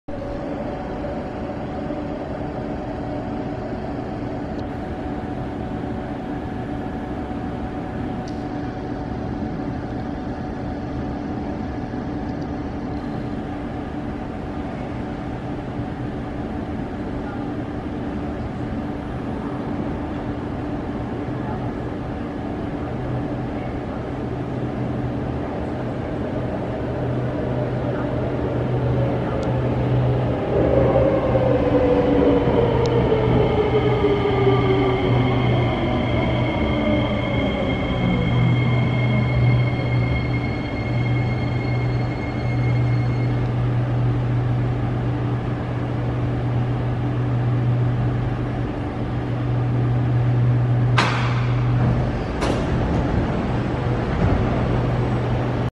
C751B 335/336 Arriving Marina South Pier (Northbound)
C751B 335/336 arriving Marina South Pier. Bound for Jurong East. 4 February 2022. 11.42 pm.